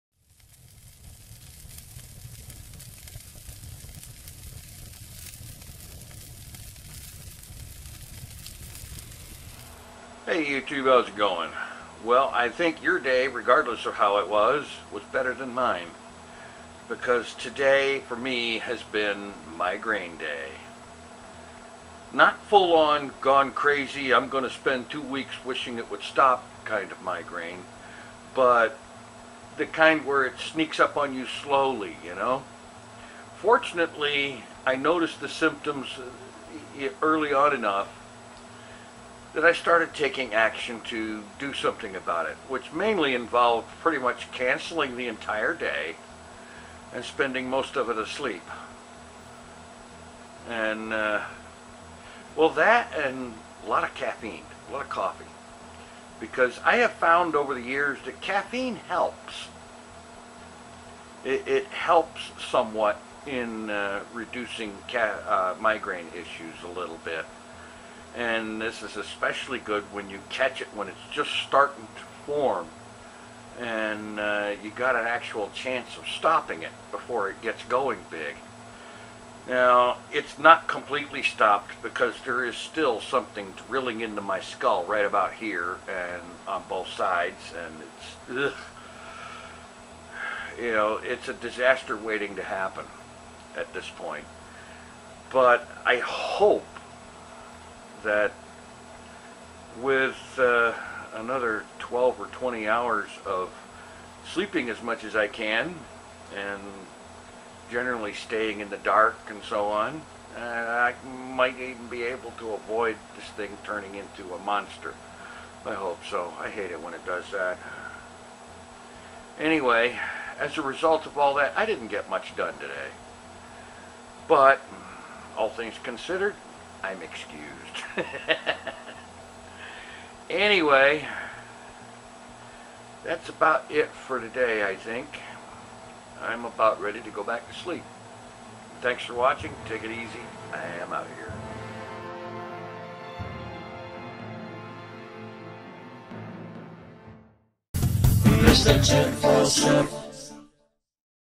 Migraine Day
[NOTE: this vlog is for 26th. its super late going live 'cause I started reading while it uploaded and forgot about it] I kinda pretty much cancelled today because of a migraine doing it's best to get started. -- 2019-65